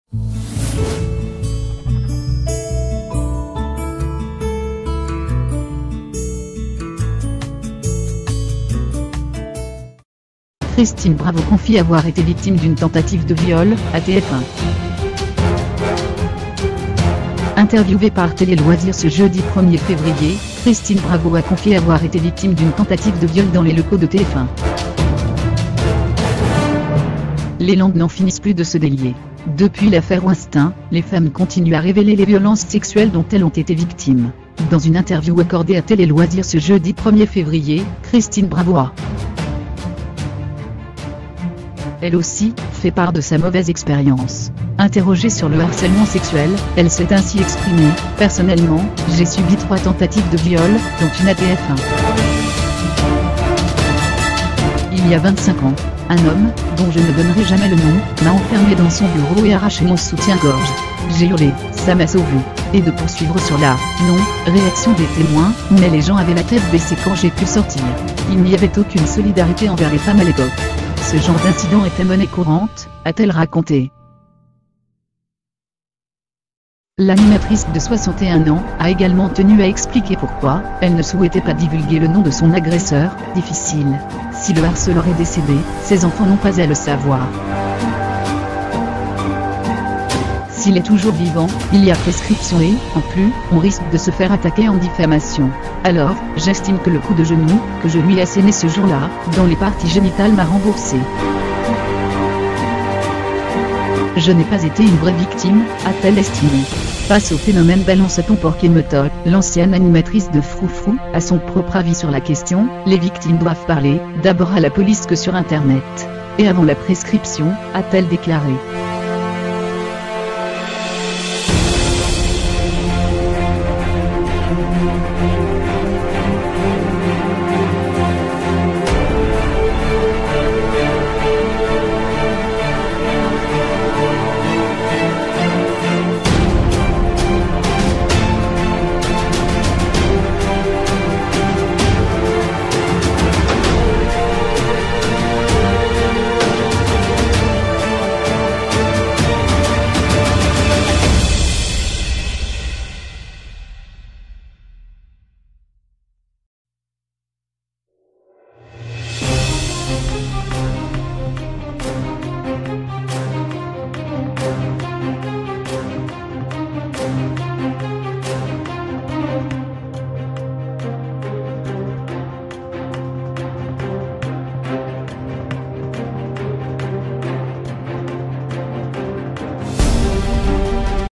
Christine Bravo confie avoir été victime d'une tentative de viol... à TF1
Interviewée par Télé Loisirs ce jeudi 1er février, Christine Bravo a confié avoir été victime d'une tentative de viol dans les locaux de TF1.